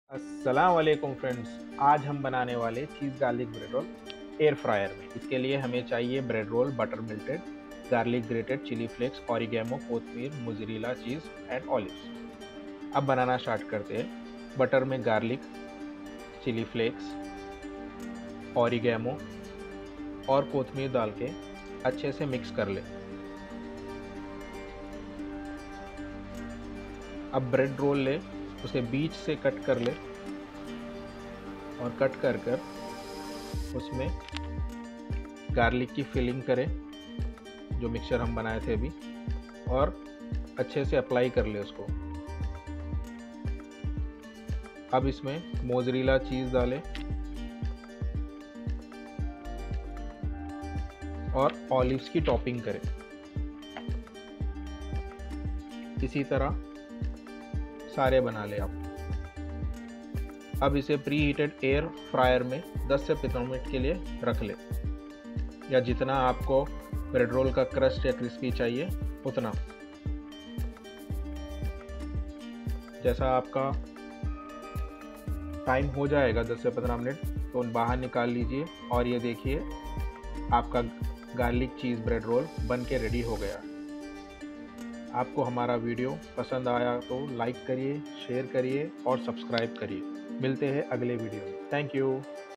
Cheese Garlic Bread in Air Fryer | Instant & Easy
Cheese Garlic Bread in Air fryer:
Ingredients:
• Bread
• Salted Butter
• Mozzarella Cheese
• Garlic (grated)
• Olives
• Dhaniya/kotmir (Coriander Leaves)
• Chili Flakes
• Oregano

Procedure:
o Add Garlic , Chili Flakes , Oregano & coriander leaves in Salted Butter and mix well
o Now Cut the Bread Roll in middle and fill it with the garlic mixture.
o Now fill the Mozzarella cheese in bread roll
o Now add topping of Olive above bread rolls.
o Pre Heat the Air fryer at 160 Deg for 10 mins.
o Now keep the prepared bread rolls in preheated Air fryer for 10-15 mins.
o Now the cheese garlic bread rolls are ready.
o Serve it with tomato sauce.